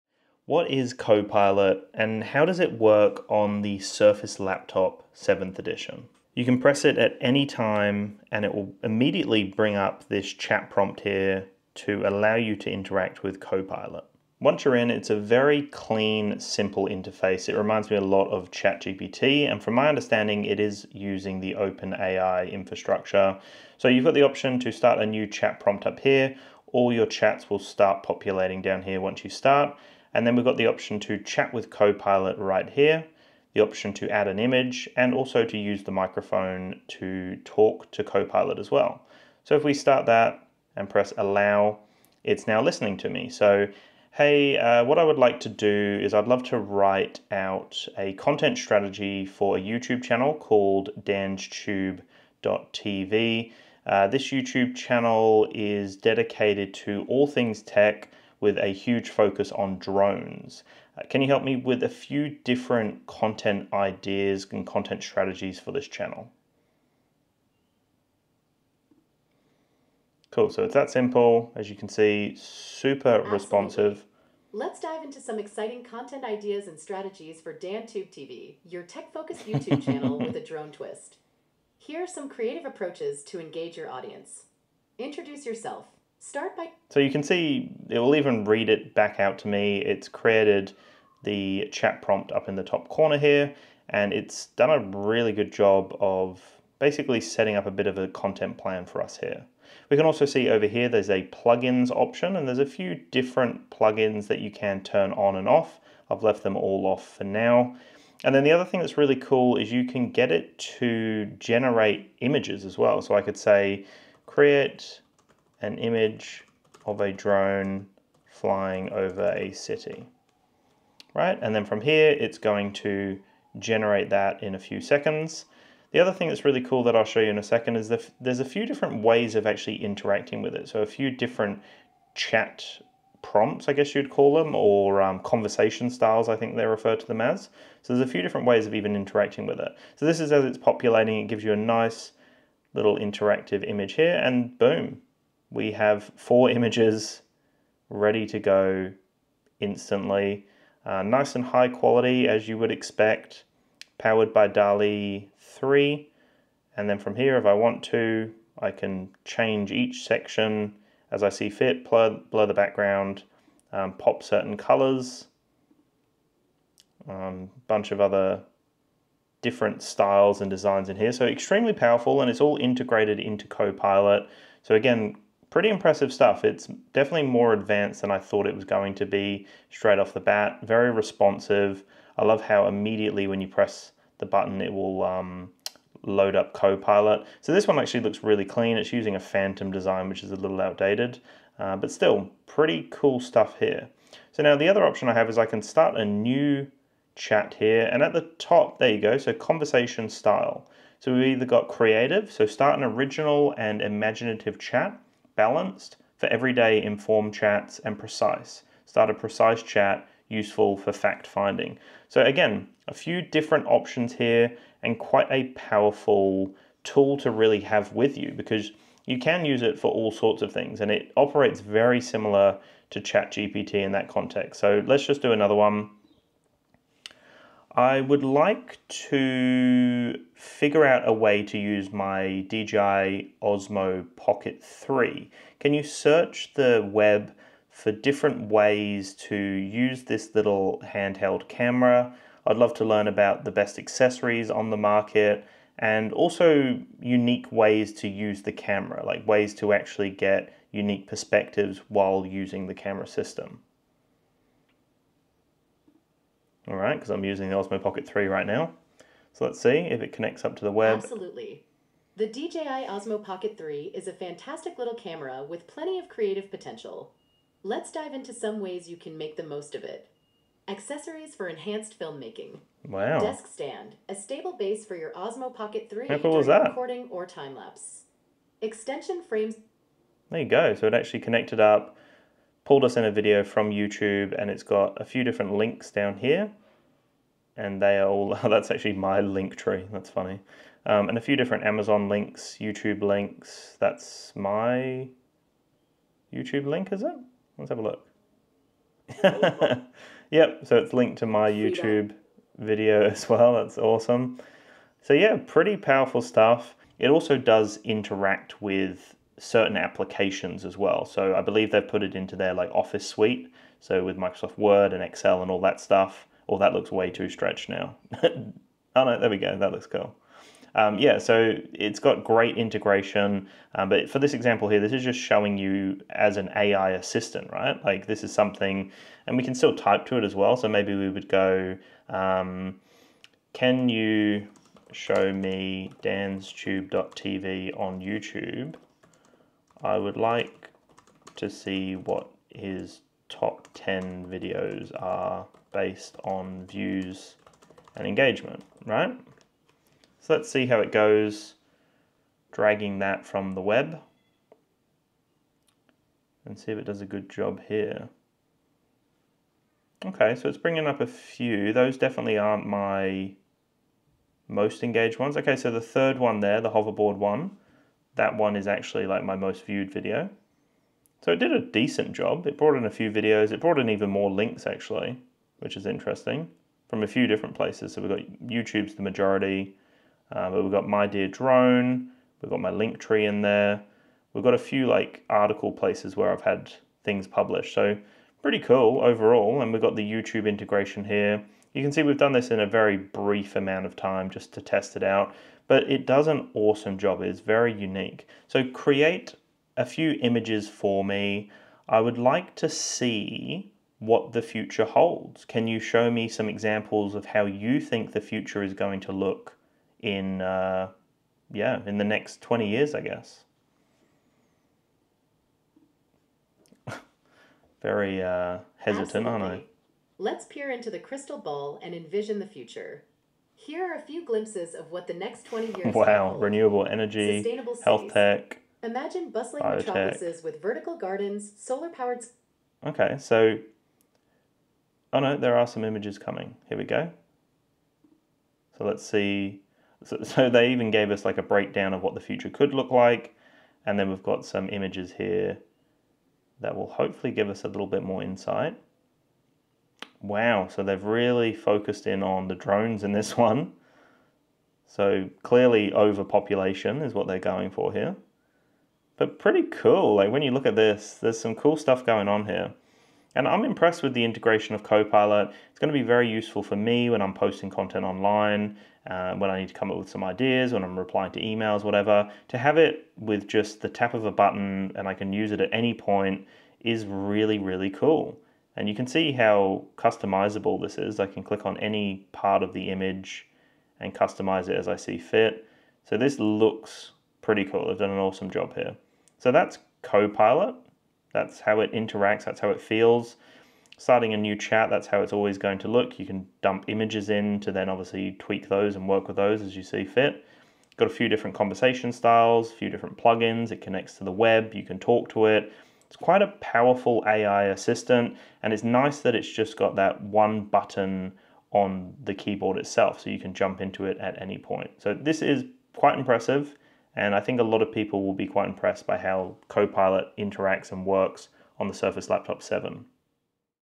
Microsoft Surface Laptop 7 (2024) - Copilot+ Explained
Copilot+ is Microsoft's latest AI-powered assistant, designed to make your life easier and your work more efficient. But how does it perform on the Surface Laptop 7 (2024)? 🤔 Exclusive tech deals